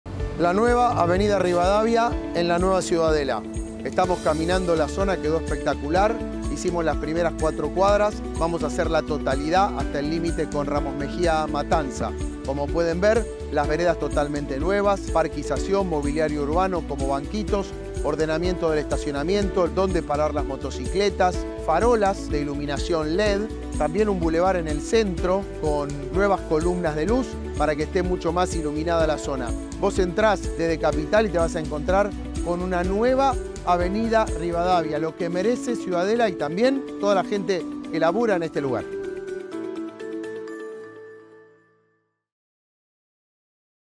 ASÍ QUEDÓ LA AVENIDA RIVADAVIA 🚧
La transformación de una arteria tan importante como la Avenida Rivadavia mejora la calidad de vida de los vecinos, potencia el trabajo de los comerciantes y trae más seguridad al barrio. Ya se encuentran completamente renovadas las primeras cuadras de este eje de circulación tan importante para Ciudadela: mejoramos las veredas, colocamos nuevo mobiliario, luminaria peatonal LED y un boulevard central para mejorar el tránsito.
Una obra integral que contempla 16 cuadras, desde la General Paz hasta la Avenida Díaz Vélez, para que las familias circulen por una zona más segura, linda y accesible. La Nueva Ciudadela, ¡no para de crecer! 💪